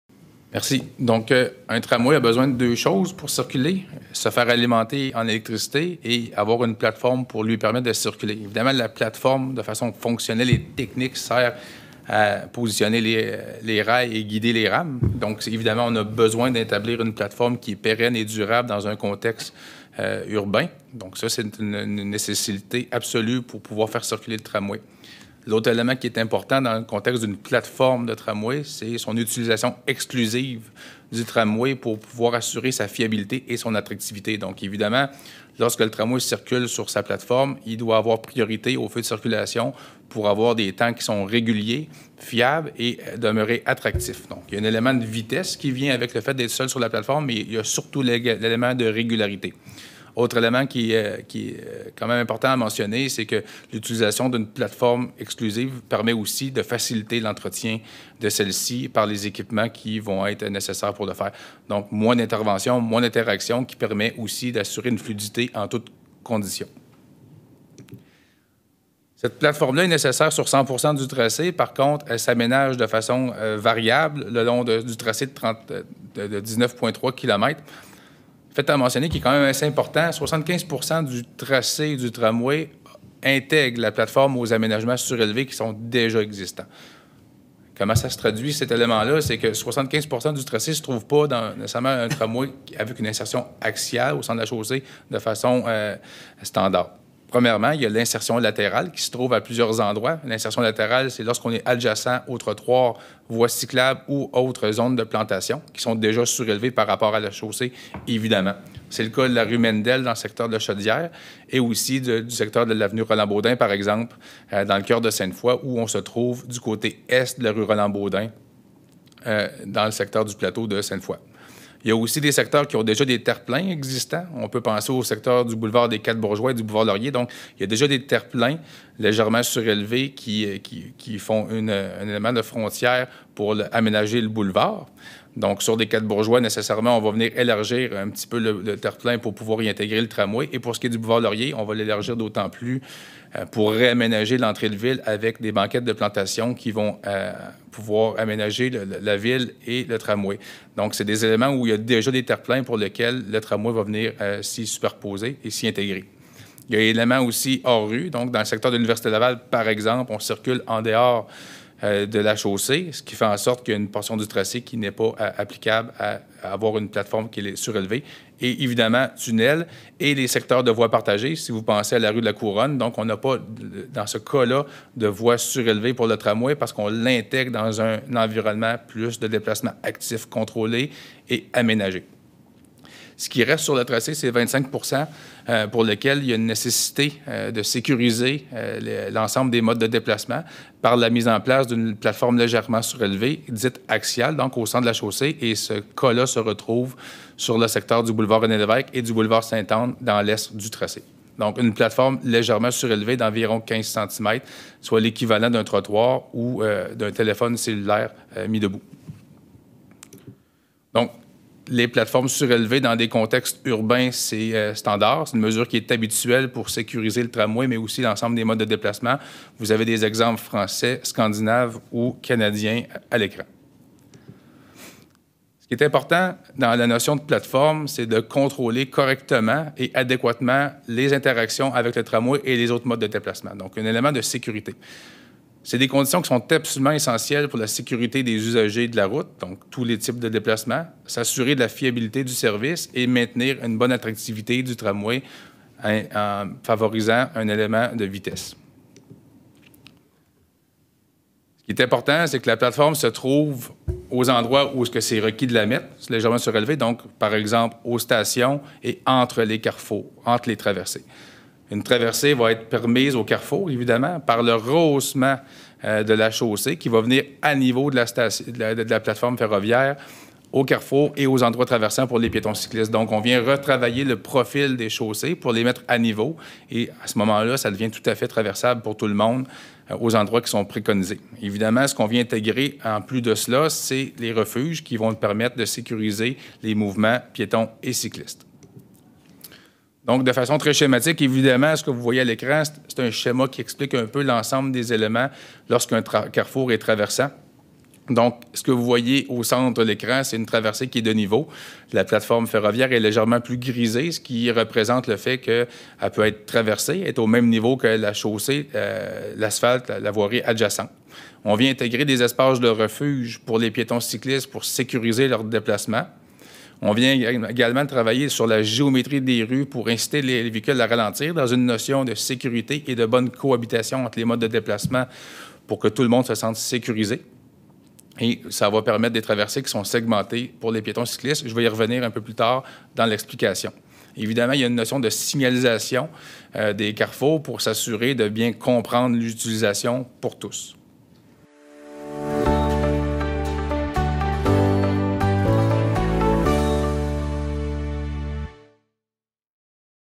Extrait 3 : La plateforme du tramway 101
Ici, on explique...
- à quoi sert la plateforme du tramway
- sur 75 % du tracé, la plateforme sera intégrée aux aménagements qui existent déjà (ex. un trottoir, d’une voie partagée ou lorsqu’il circule en dehors de la rue ou en tunnel)
- des exemples d’intégration dans le monde
- à quoi ressemblera un carrefour traversant.